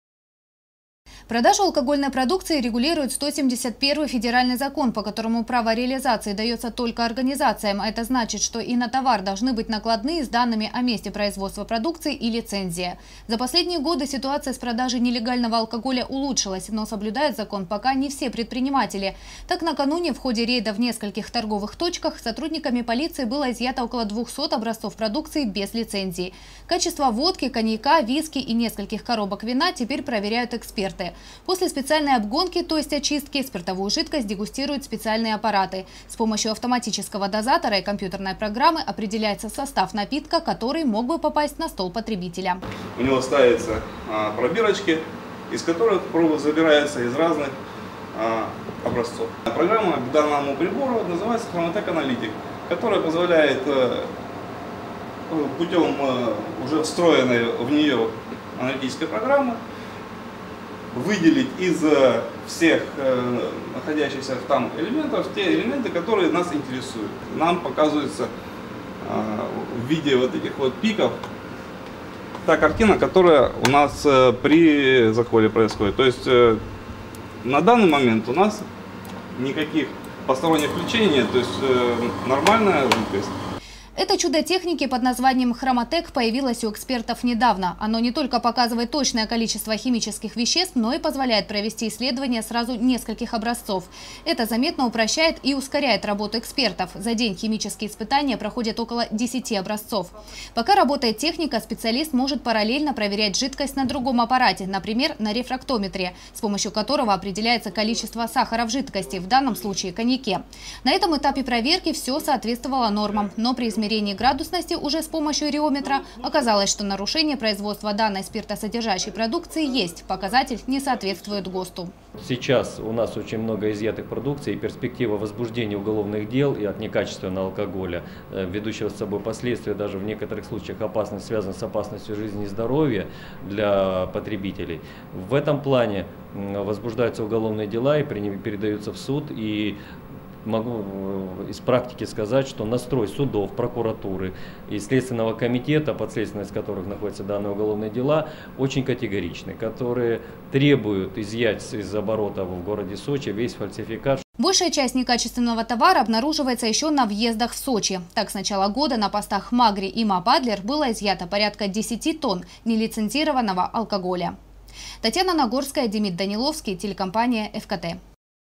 За последние два месяца изъято порядка 10 тонн нелегальной алкогольной продукции. Новости 24 Сочи
Поступление такого количество контрафакта в торговлю удалось предотвратить на пунктах пропуска Адлер и Магри. Тем не менее, у некоторых реализаторов все ещё имеется продукция без необходимых документов и акцизных марок. Так накануне, сотрудники полиции изъяли около 200 таких образцов алкоголя. Они сейчас проходят экспертизу.
Сочи. Новости.
Телеканал Эфкате.